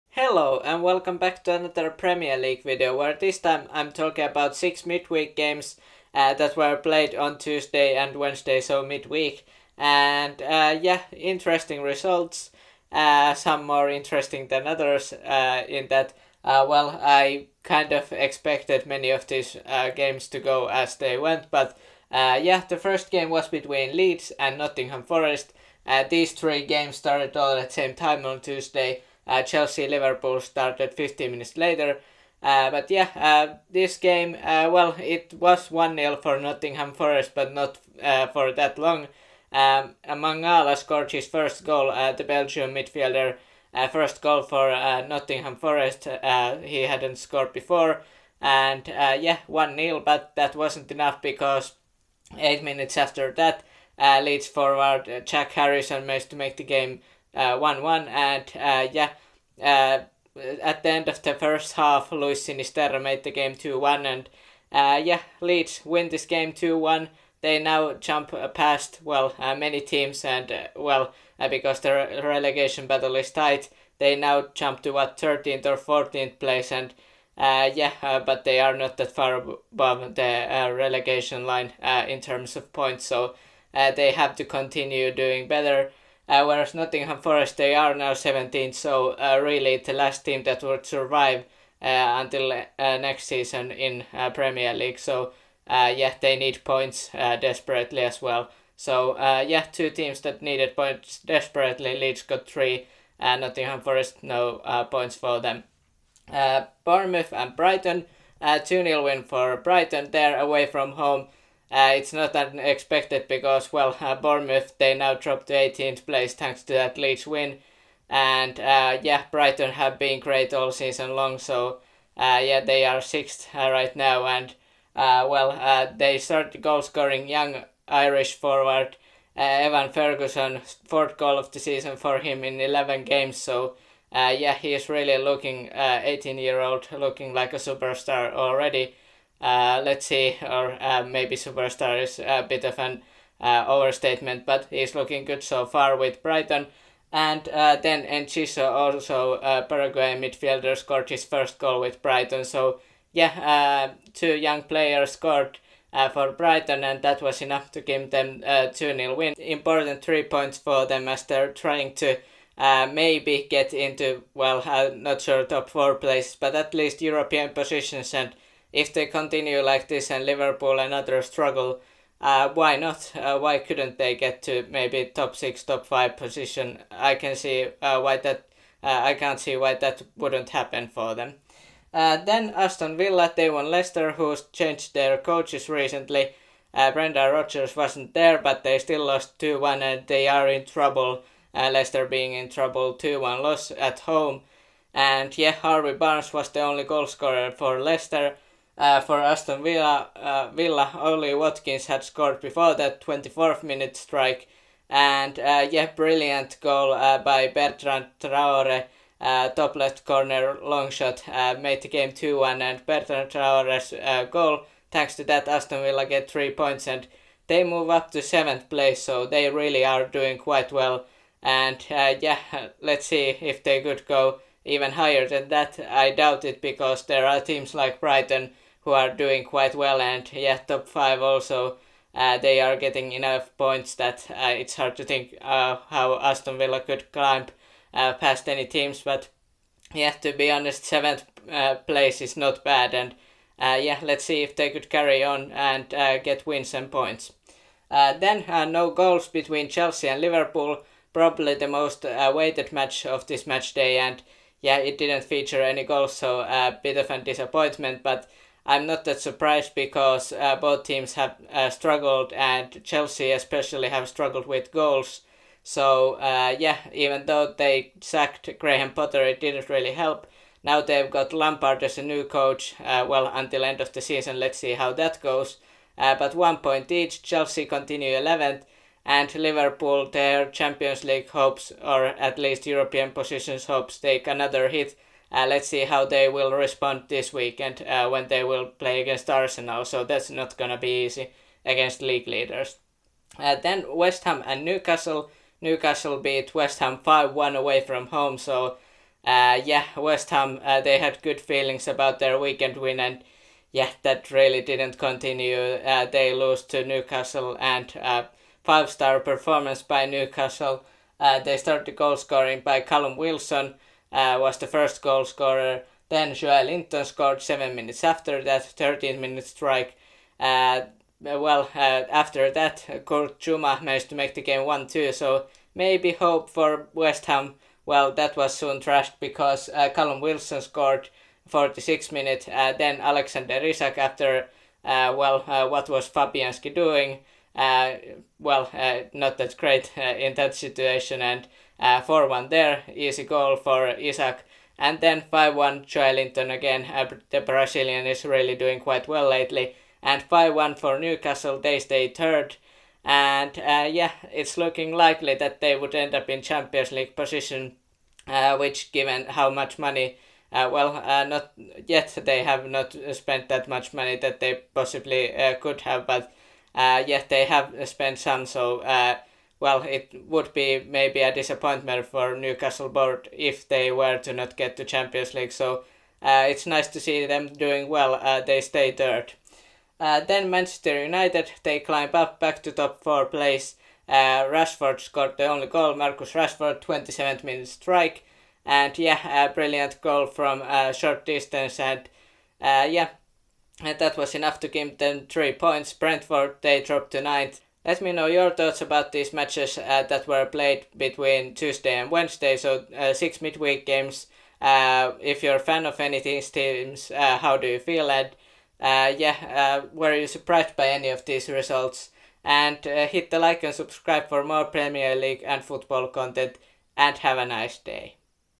Reviewing Premier League Midweek Games
My summary of Premier League midweek games. What do you make of the results?

Timestamps:
0:00 Intro
0:23 Leeds vs Nottingham Forest, Bournemouth vs Brighton
3:02 Leicester vs Aston Villa, Chelsea vs Liverpool
4:53 West Ham vs Newcastle, Man United vs Brentford
6:38 End thoughts